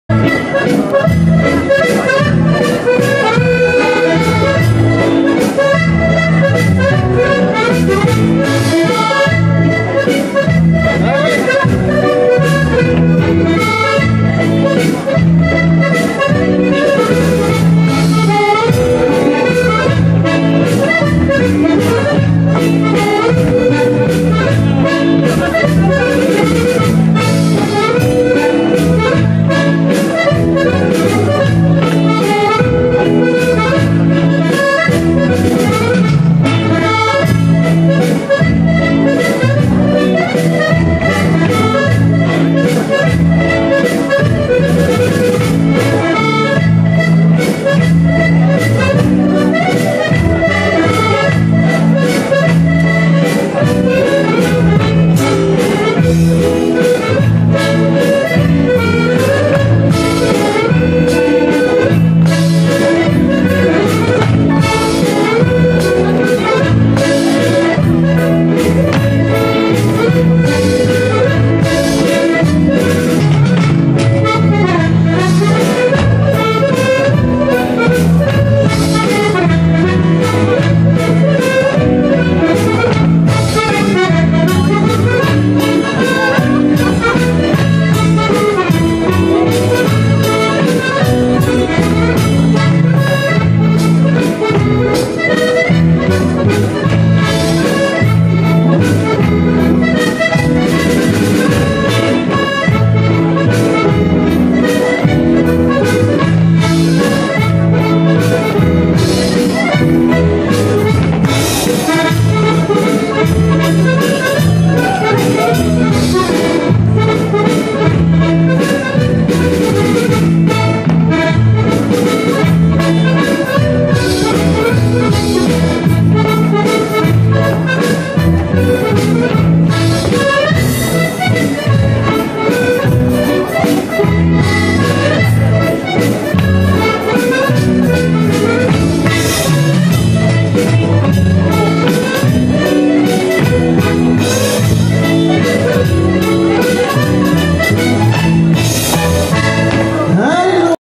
Jacques Granié.............mazurka
Gala d'accordéon au dancing "au bon temps" à Aurillac (15)
animé par l'orchestre de Claude Meallet
le 27 mars 2015
réalisation vidéo : Martine Séméteys (15accordeon)